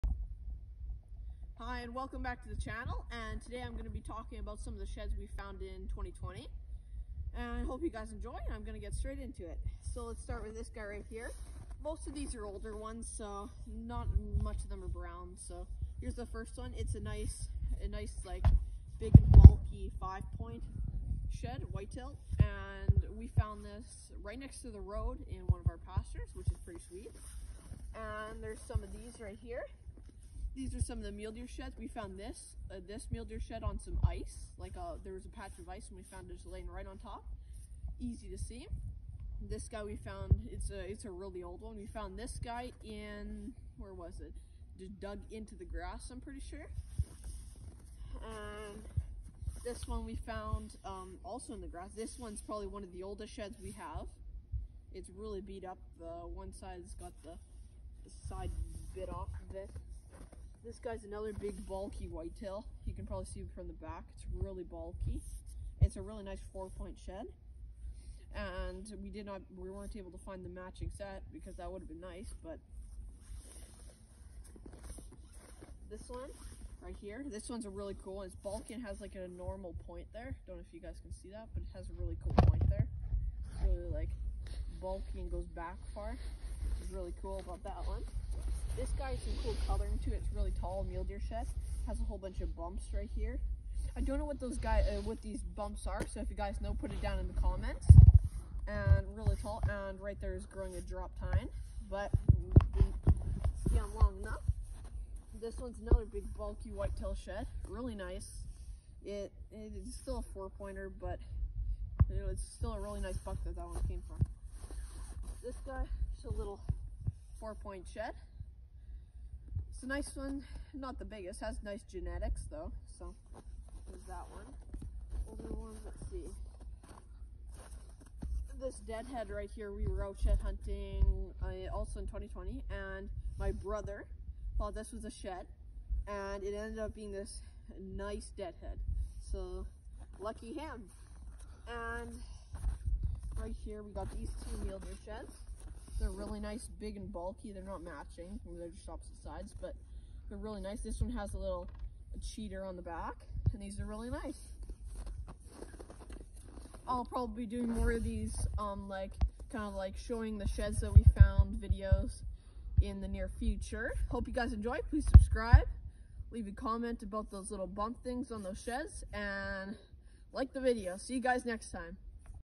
2020 sheds!
Shed hunting is a really big passion of mine so you guys will be seeing a lot more videos on shed hunting! Hope you enjoy!